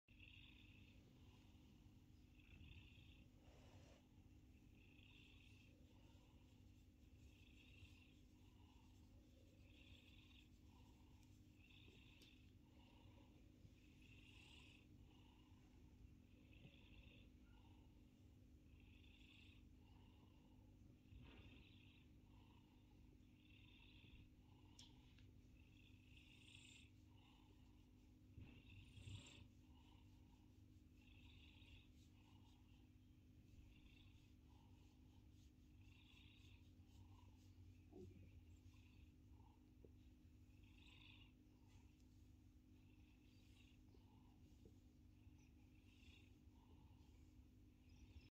Monty go brrrrrr
In which our hero shows off his (very quiet) engine noises.